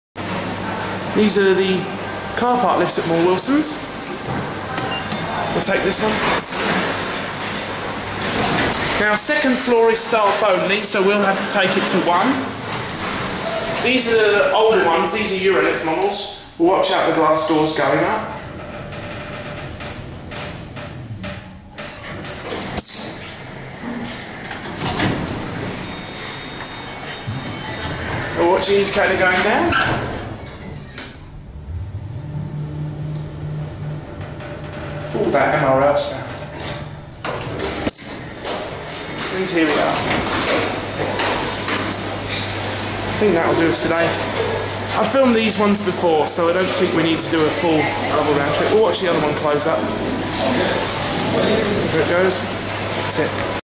Schindler 2400 MRL Car Park Lifts - Moore Wilson's Tory Street.
A pair of nice Schindler MRL's with FI MXB buttons and glass Doors. The 2nd Floor is the staff car park, which is why it was locked off.